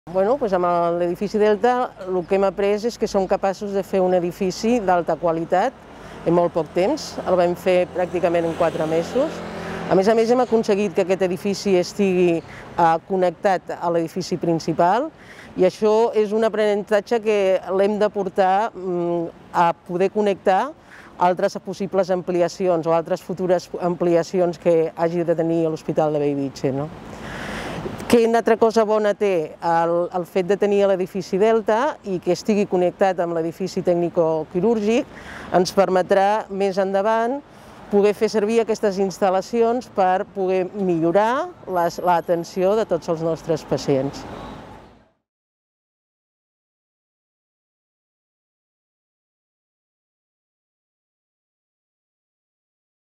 Què hem après en aquests 2 anys de pandèmia? Hospital Universitari de Bellvitge (2/6)
La Dra. Shaw, Subdirectora del Procés d’Hospitalització i Gestió de Pacients del nostre hospital, explica l'impacte de la construcció de l'Edifici Delta, destinat a la cura de pacients